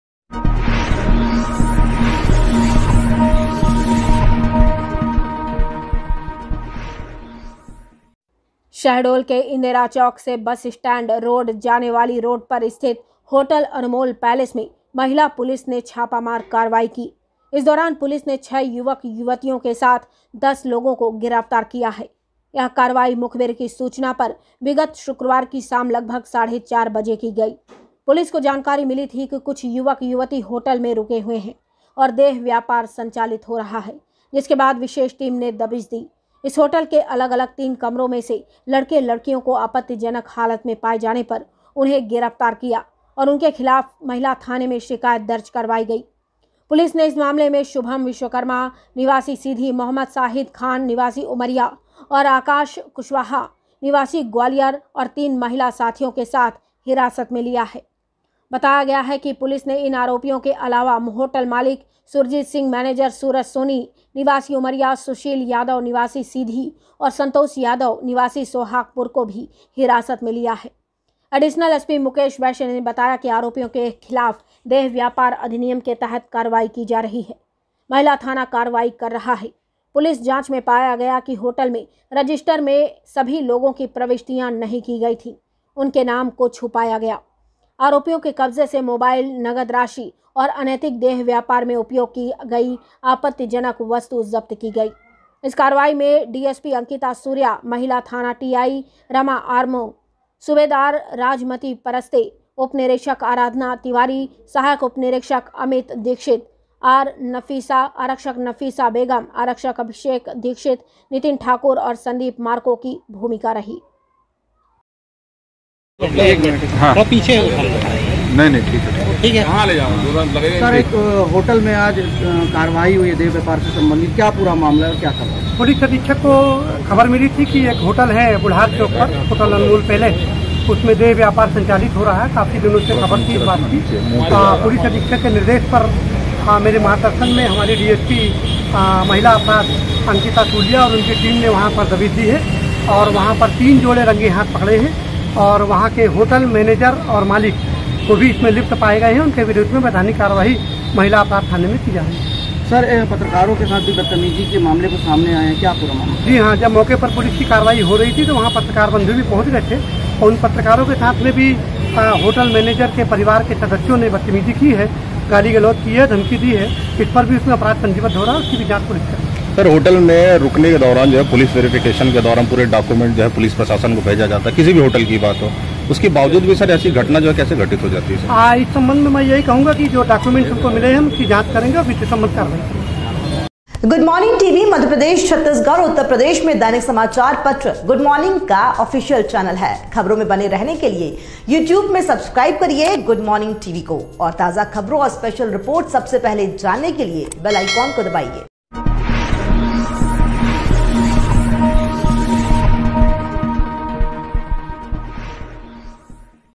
Shahdol | होटल अनमोल पैलेस में पुलिस का छापा, आपत्तिजनक हालत में मिले 6 युवक-युवती | Good Morning TV
शहडोल शहर के बीचोंबीच स्थित होटल अनमोल पैलेस में पुलिस का छापा, आपत्तिजनक हालत में मिले 6 युवक-युवती। होटल मालिक सहित 10 लोगों को पुलिस ने किया गिरफ्तार। होटल अनमोल पैलेस में लंबे समय से देह व्यापार की पुलिस को मिल रही थी शिकायत।